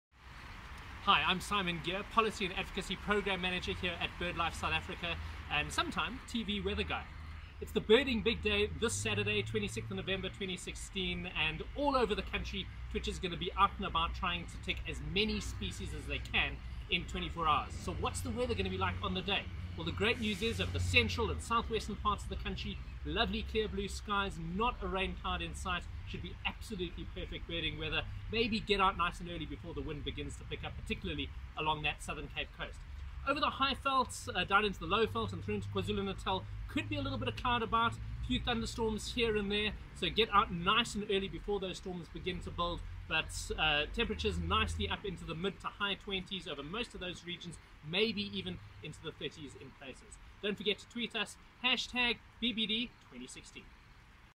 Simon Gear presents the Weather for BBD2016
Simon Gear, South Africa’s famous weatherman, and BirdLife South Africa’s Policy & Advocacy Programme Manager, provides this Birding Big Day’s weather forecast. Remember to hashtag